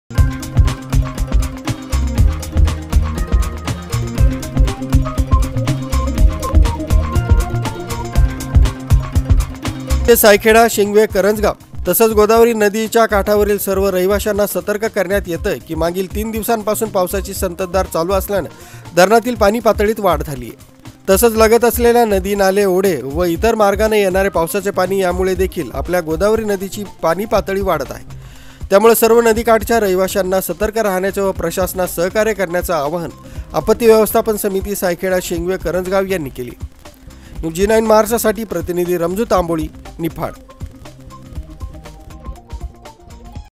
नांदूरमध्यमेश्वर धरणातून 6310 क्युसेक पाण्याचा विसर्ग..गोदावरी नदीची पाणी पातळी वाढ..
न्युज जी९ महाराष्ट्रसाठी निफाड प्रतिनिधी रमजू तांबोळी